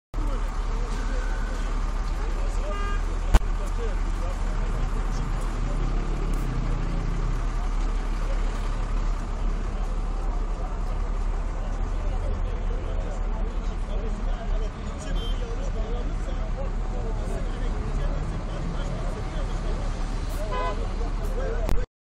Siverek Haber Haber Kanalı
Siverek'te seyir halinde olan bir araç aniden alev alarak yandı.